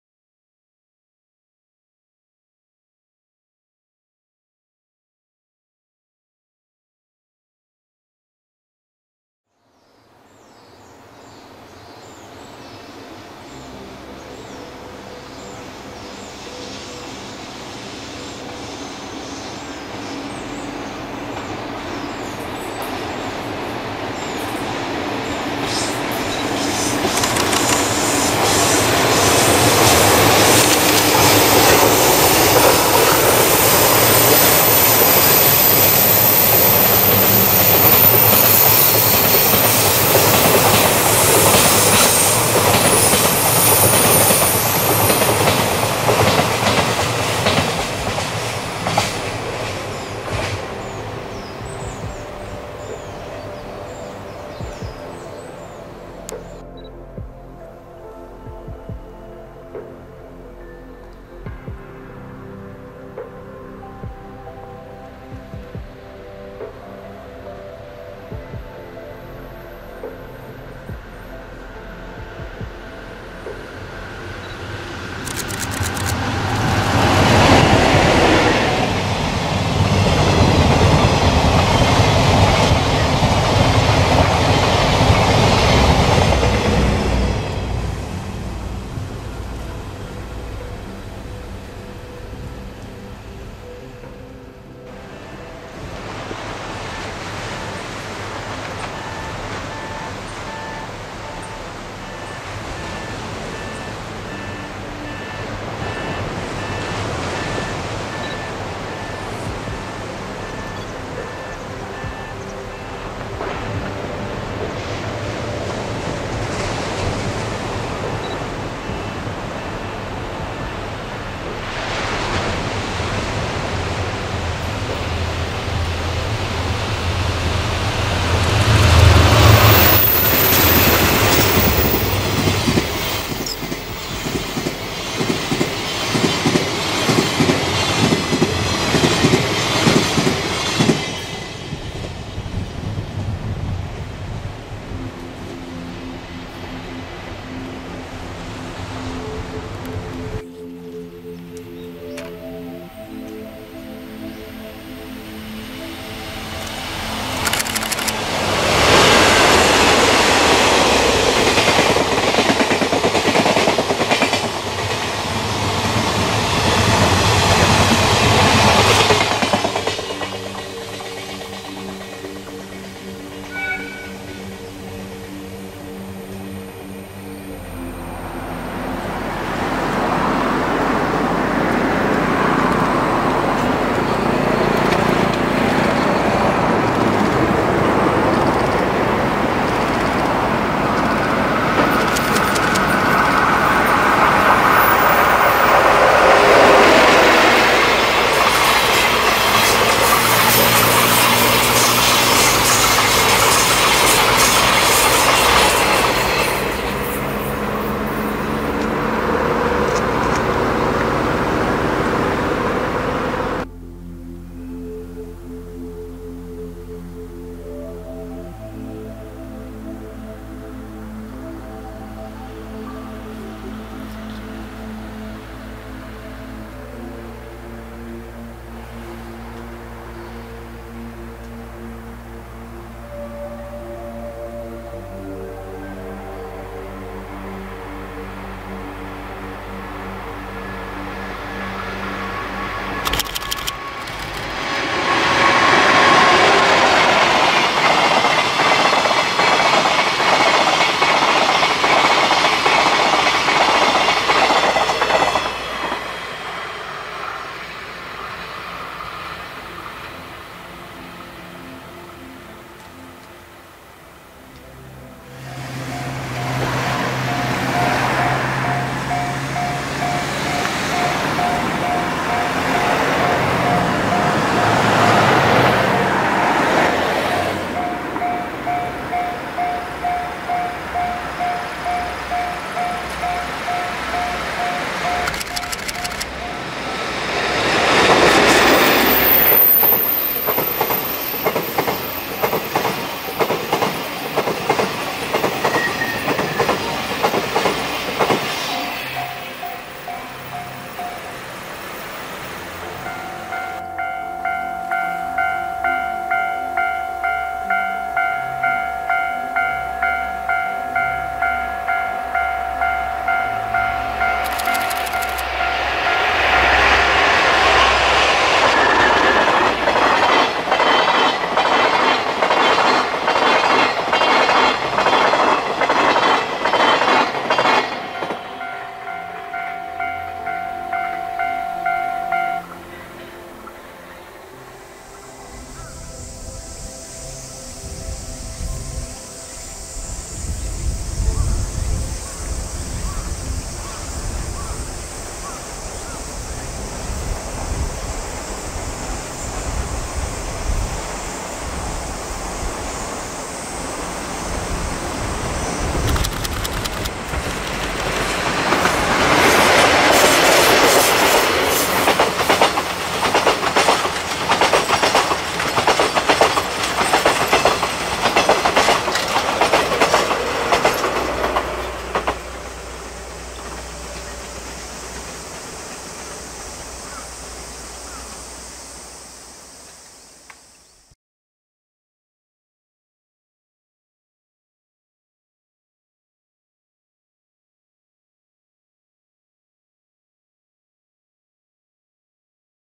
トワイライトエクスプレス号撮影動画(Twilight Express Train Movie.)
2011年から2015年にかけて撮り溜めた寝台特急「トワイライトエクスプレス」号の映像をつなげて1つの動画にしてみました、、、、